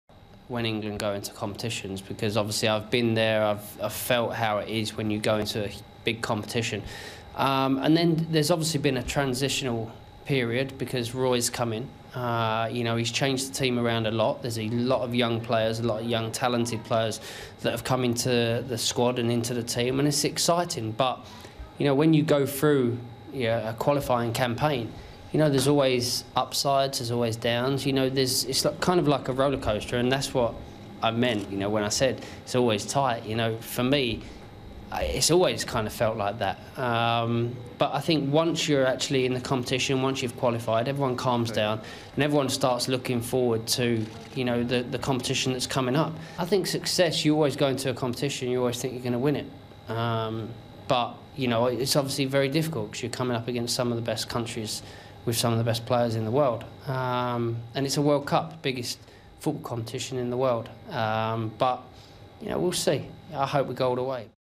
► David Beckham :" England's World Cup chances and remains hopeful "
David Beckham gives his opinion on England's World Cup chances and remains hopeful they can go the whole way.